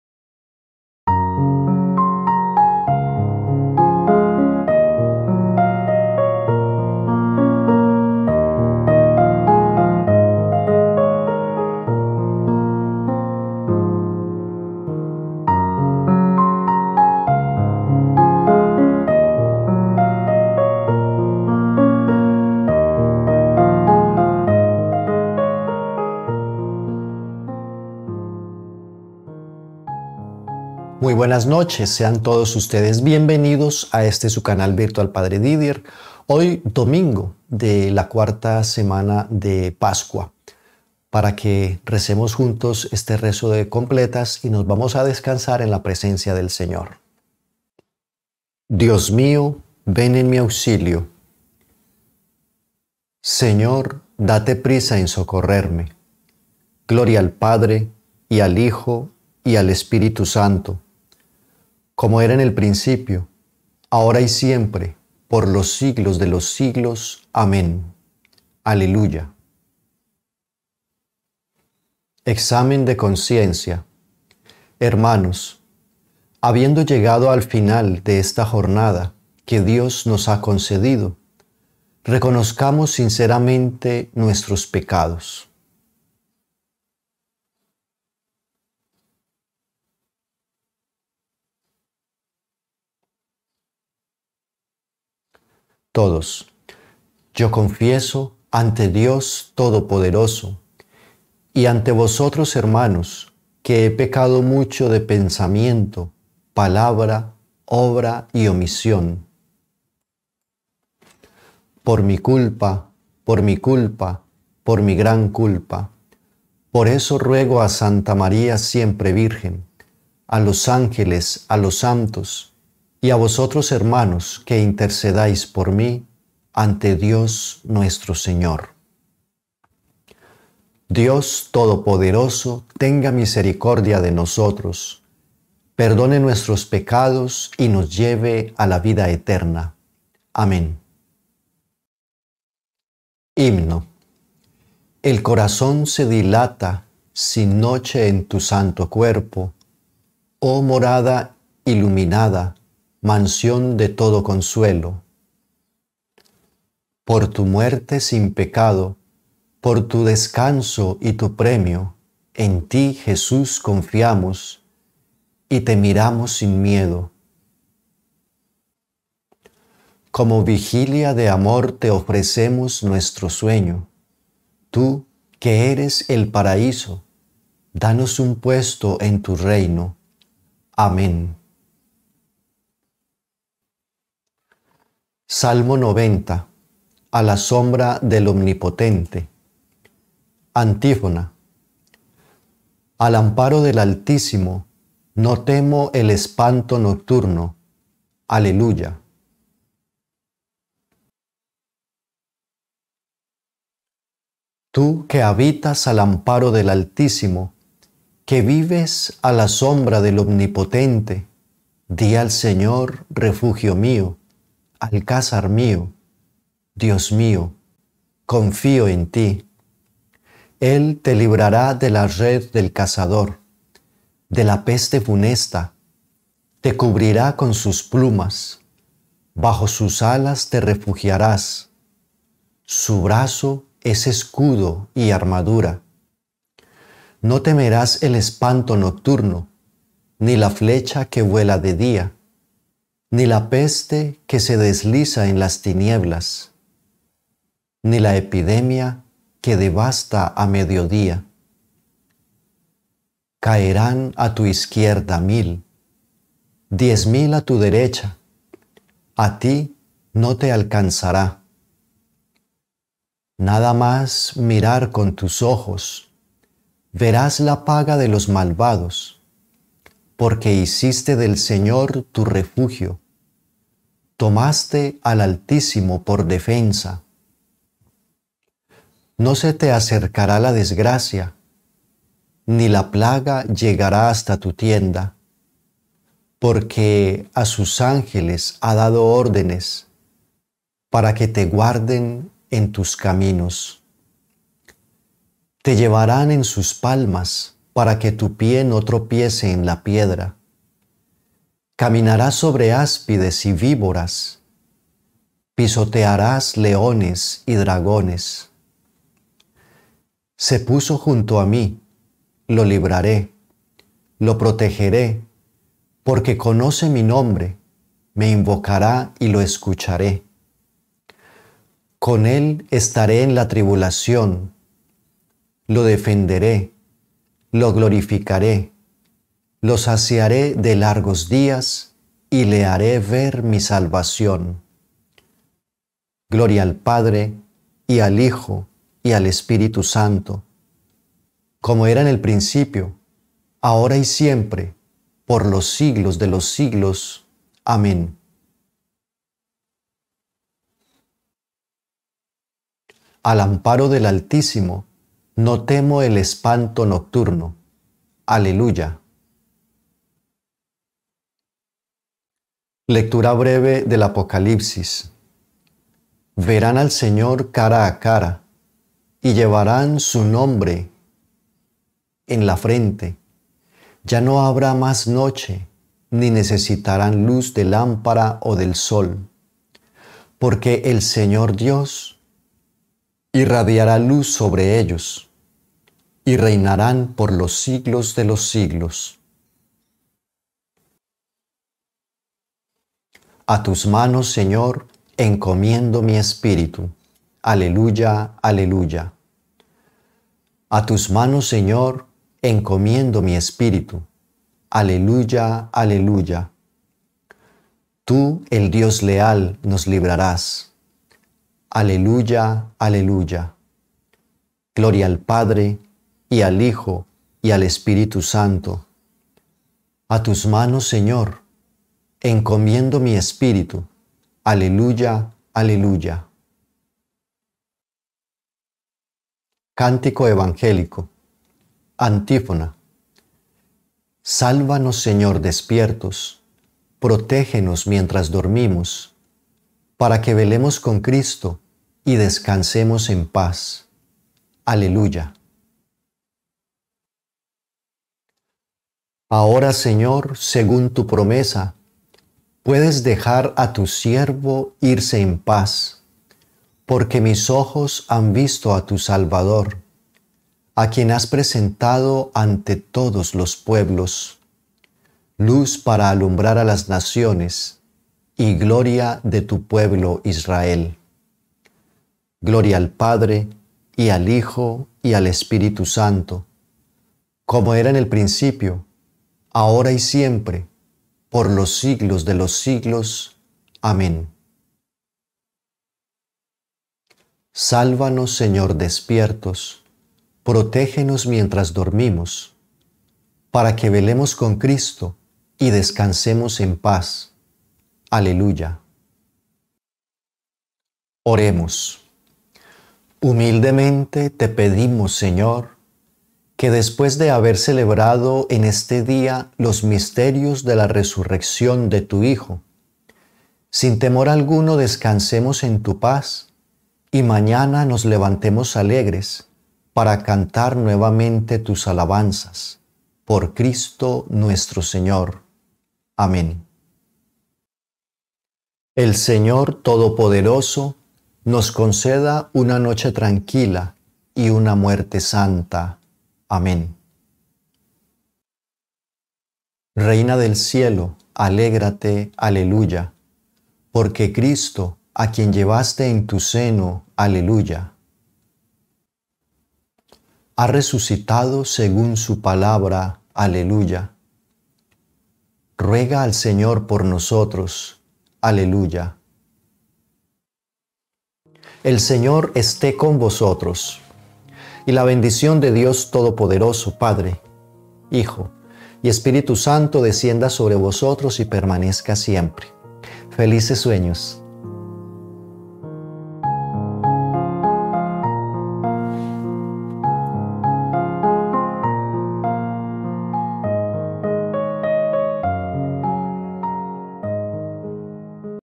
Completas de hoy Domingo 21 Abril 2024. Oración de la noche. Padre Didier
Liturgia de las horas
Completas de hoy